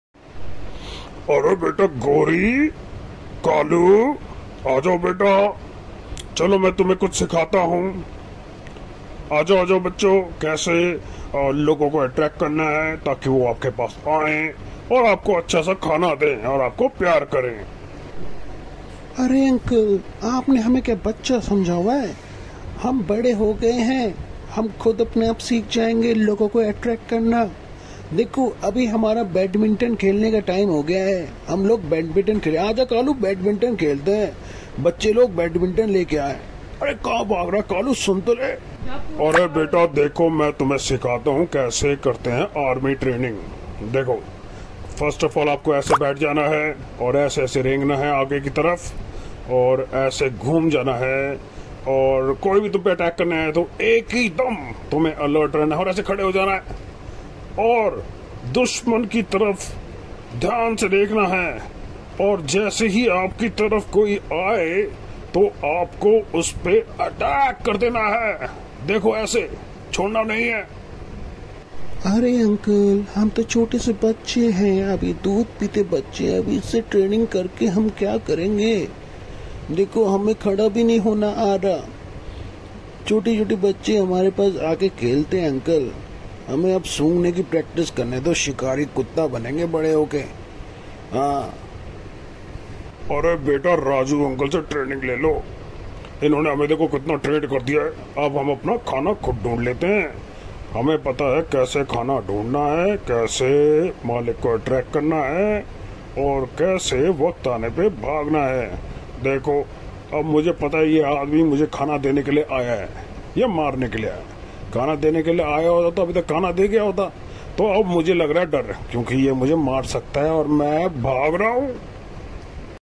कालू trainer uncle aur बच्चे || streetdogs puppystory bacho ke liye 🤣😝 🐕🐶
कालू ट्रेनर अंकल और बचे
First 2 min dog vlog for kids
Street dog vlog - Trainer kaalu and puppies
Kaalu Trainer Uncle aur Bache
The Journey of a Street Dog" 🐾
checkout the incredible story of a street dog trainer journey to teach street puppies. This heartwarming 2-minute video showcases the resilience and strength of stray dogs.